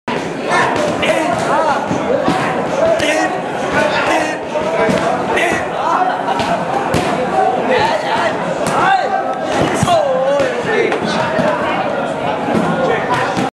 Финишные удары в тренировке. Силы покинули.
Финишные удары в тренировке. Силы покинули. Тайский бокс. Тайгер, Пхукет, Таиланд. Михаил Шилов